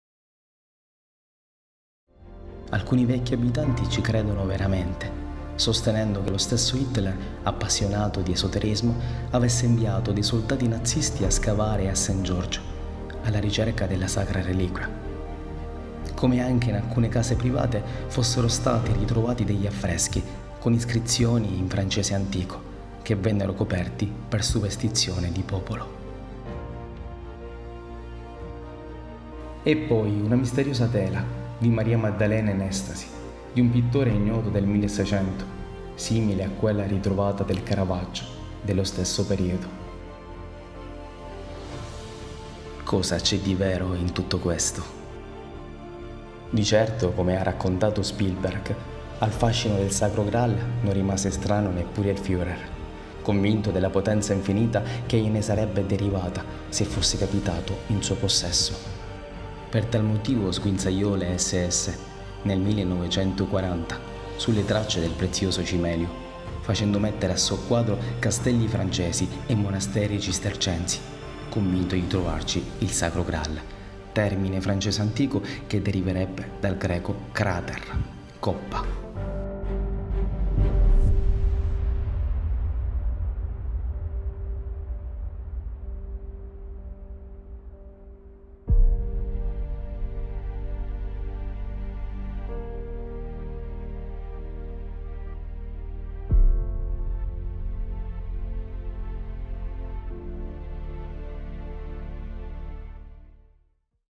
LA MADDALENA RITROVATA - Visit Gratteri
Uno dei tesori che custodiscono le chiese di Gratteri, è stato riscoperto di recente, ritornato a far parlare di sé dopo un lunghissimo oblio. Si tratta di una tela di Santa Maria Maddalena in estasi di un ignoto scultore del 1600 che ultimamente sta facendo parecchio discutere poiché risulterebbe essere simile ad un’altra inedita, ritrovata qualche anno fa e attribuita a Michelangelo Merisi detto il Caravaggio. Era l’ottobre del 2014, quando la storica dell'arte Mina Gregori rese noto il ritrovamento - in una collezione privata europea - di una Maddalena, da lei riconosciuta come l'originale del Caravaggio sulla base dell'analisi tecnico-stilistica e la presenza di un biglietto, con grafia seicentesca, ritenuto autografo del Merisi che riportava la scritta "Madalena reversa di Caravaggio a Chiaia ivi da servare pel beneficio del Cardinale Borghese di Roma". L’opera – chiamata oggi "Maddalena Klain", dal nome dei penultimi proprietari privati - è un dipinto a olio su tela (cm 106,5 x 91) attribuito a Caravaggio e databile attorno al 1606, anche se alcuni critici, tra cui John Spike, propendono per il 1610.